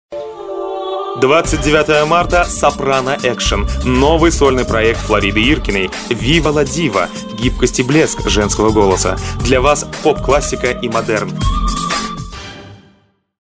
Фларида Иркина
29 марта. Сопрано-экшн. Новый сольный проект Флариды Иркиной! ВИВА ЛА ДИВА! Гибкость и блеск женского голоса. Для вас – поп-классика и модерн! Телефон для справок: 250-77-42, категория: 6+